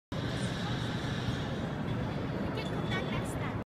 Tornado at Science & Industry Museum
The tornado turning off at closing time at the Science and Industry Museum in Chicago. "Please come back next time!"